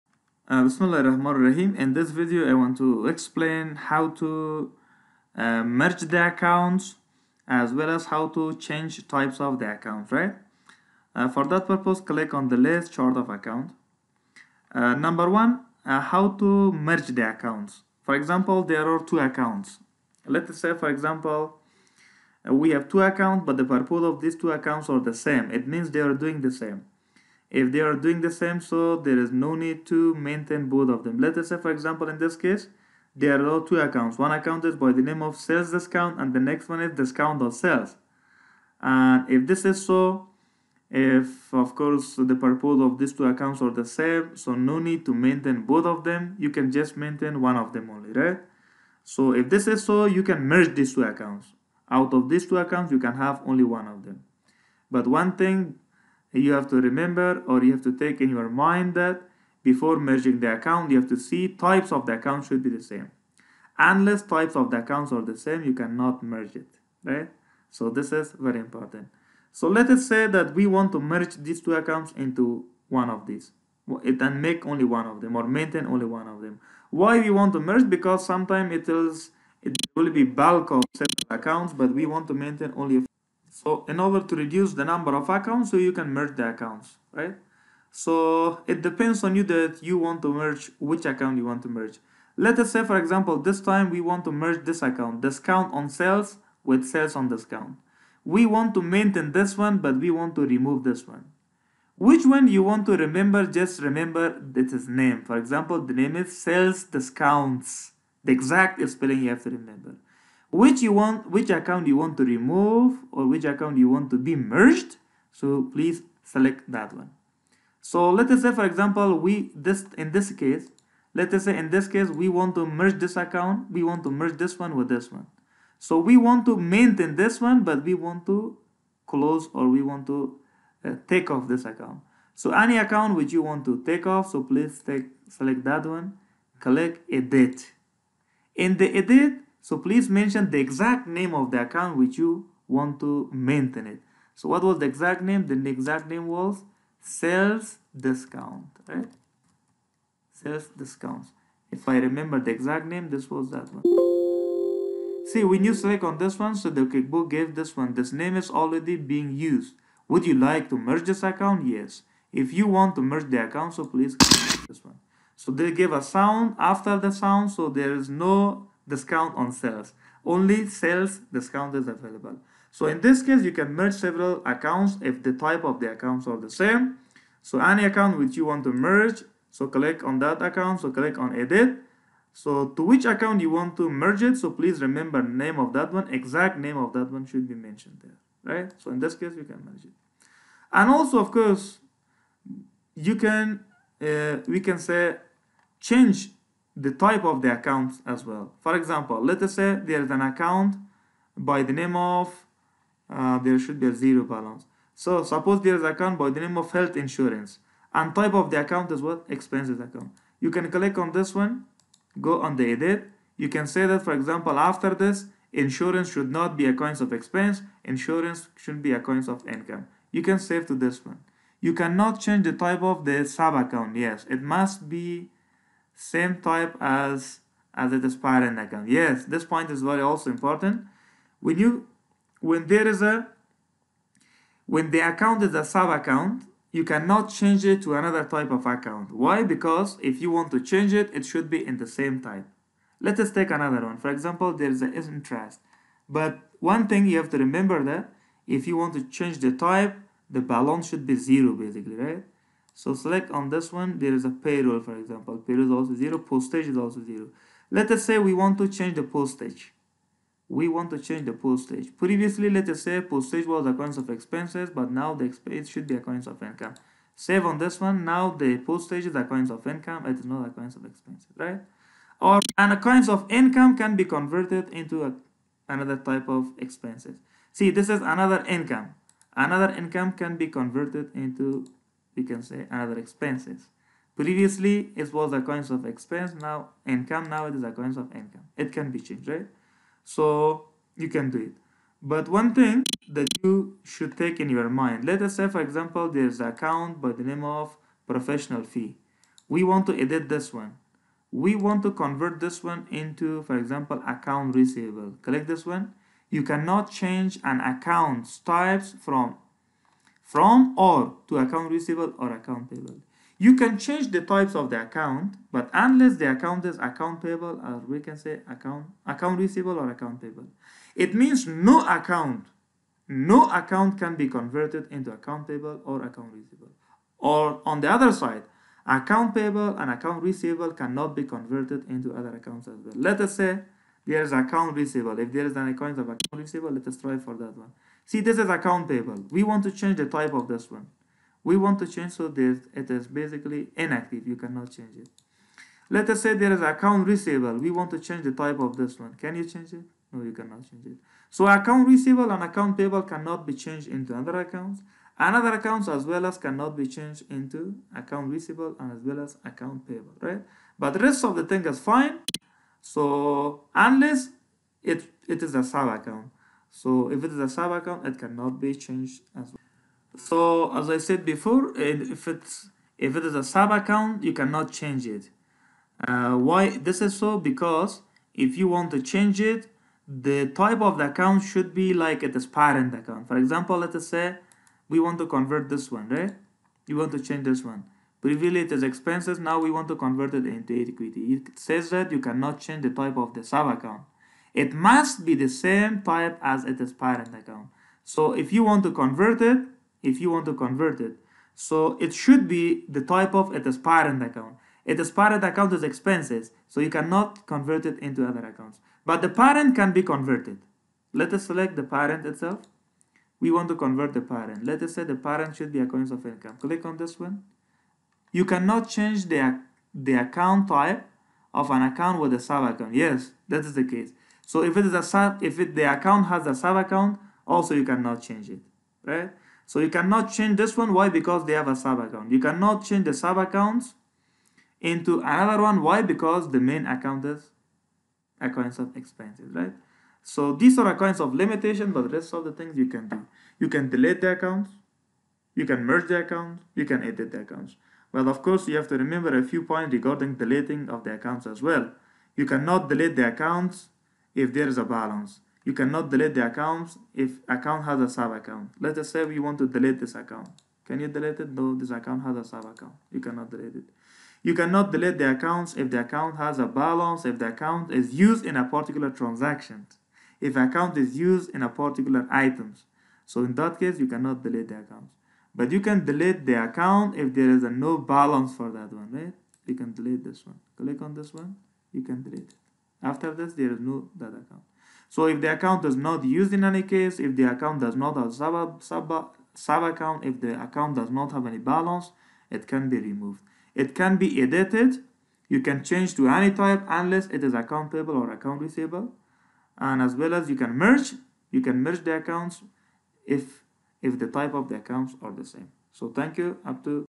Lesson 3: How to Edit Chart of Accounts in QuickBooks
this video explains the steps involved to merge two accounts, delete the accounts and modify types of accounts in the chart of accounts.